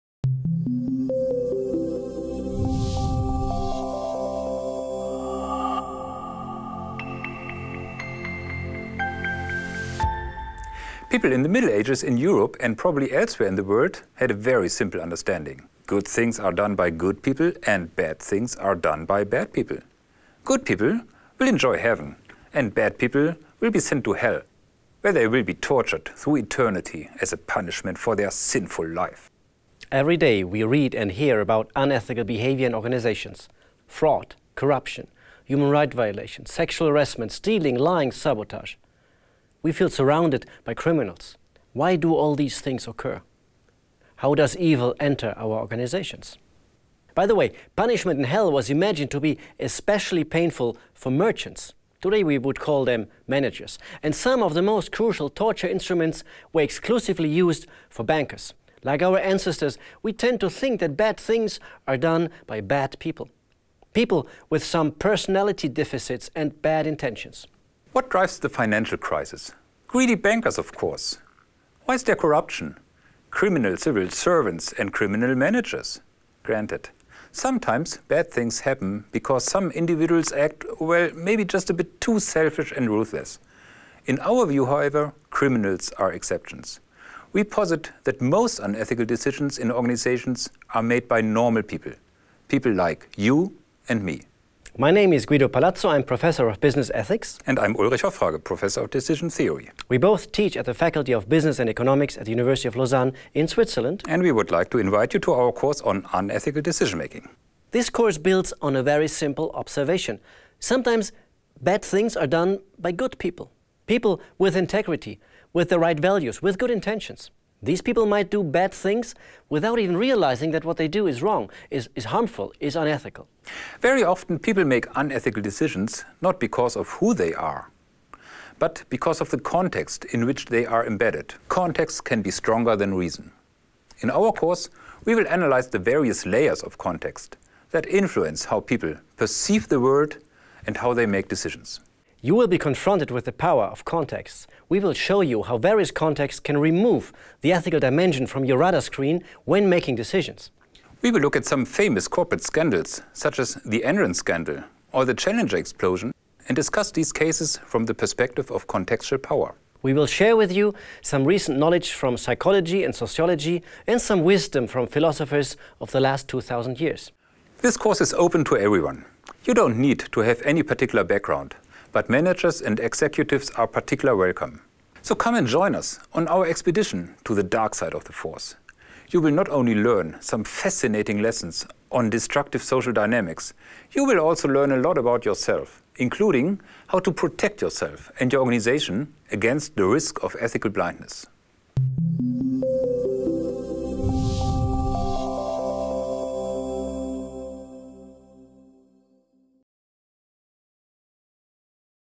Unethical decision making in organizations - UNIL MOOCS
La prise de décisions contraire à l'éthique dans les organisations
Unethical decision making in organizations

Guido Palazzo
Professor of Business Ethics
Faculty of Business and Economics (HEC-Lausanne)
University of Lausanne

Ulrich Hoffrage
Professor of Decision Theory
University of Lausanne

This course 'Unethical decision making in organizations : A seminar on the dark side of the force' will teach you how strong organizational contexts push good people towards unethical decisions. You will also learn how to protect yourself and your organization against such forces lurking in the dark.

Regarder la vidéo d'introduction
À propos du cours
Whenever we hear about ethical scandals, we tend to believe that unethical or illegal behavior in organizations is driven by character deficiencies of individual actors. Put differently, we simply assume that bad things are done by bad people (so-called bad apples). However, numerous corporate scandals, such as Enron, Ford, or Siemens, have demonstrated that even people with a high level of integrity can break the rules if they are put into a corrupt context. Good apples may become rotten in bad barrels. Regardless of their good intentions and strong values, individual actors might adapt to the unethical practices in their respective organizational context and, over time, lose the ability to see their wrongdoings. They become ethically blind. Such changes in people’s way of seeing the world and their value systems can occur in—and through—various organizational contexts in business, civil society, and public administration. This course is not about apples but about barrels and barrel makers. A better understanding of why and under what conditions good people make bad ethical decisions will enable us to better protect individuals as well as their respective organizations against the overwhelming power of the context. It will also enable us to cure societies from problems like corruption.

The goal of this course is to empower students to analyze the risks of unethical or illegal behavior that might be triggered by powerful contexts. It will draw from various disciplines such as management, psychology, sociology, philosophy, and literature, in order to learn what these disciplines contribute to a better understanding of unethical behavior. The course will also analyze some of the most eminent organizational scandals of the recent decades through the lenses of these disciplines.